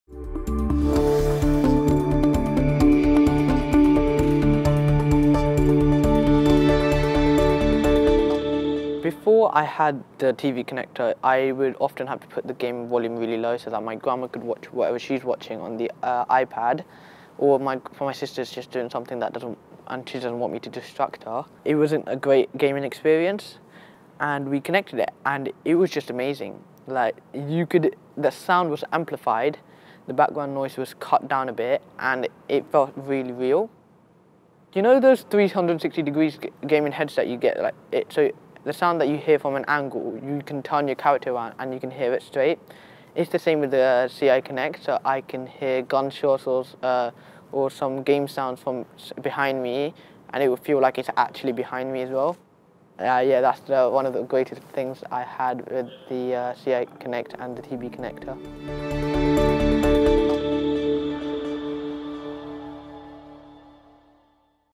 Teen streams video game audio directly to Bluetooth enabled sound processor
Seavahan is 15 years old and has been bilaterally implanted for the past 8 years or so.   He uses the Naída CI Connect with a device called the TV Connector, which is a small device that is attached to the TV or computer to direct stream the sound to the CI.  Seavahan uses this technology for online gaming instead of disturbing his family by playing the sound from speakers or wearing a bulky headset.  He describes the advantages it brings him.